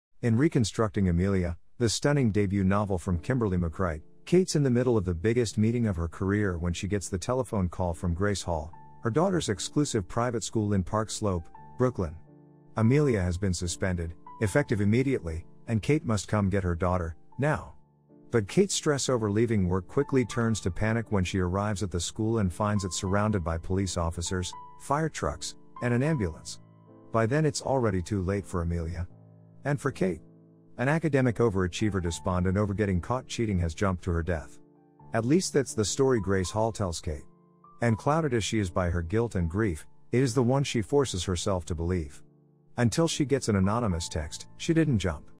Book review Summary of Reconstructing Amelia by Kimberly McCreight
In Reconstructing Amelia, the stunning debut novel from Kimberly McCreight, Kate's in the middle of the biggest meeting of her career when she gets the telephone call from Grace Hall, her daughter’s exclusive private school in Park Slope, Brooklyn. Amelia has been suspended, effective immediately, and Kate must come get her daughter—now. But Kate’s stress over leaving work quickly turns to panic when she arrives at the school and finds it surrounded by police officers, fire trucks, and an ambulance. By then it’s already too late for Amelia. And for Kate.
An academic overachiever despondent over getting caught cheating has jumped to her death. At least that’s the story Grace Hall tells Kate. And clouded as she is by her guilt and grief, it is the one she forces herself to believe. Until she gets an anonymous text: She didn’t jump.
Reconstructing Amelia is about secret first loves, old friendships, and an all-girls club steeped in tradition. But, most of all, it’s the story of how far a mother will go to vindicate the memory of a daughter whose life she couldn’t save.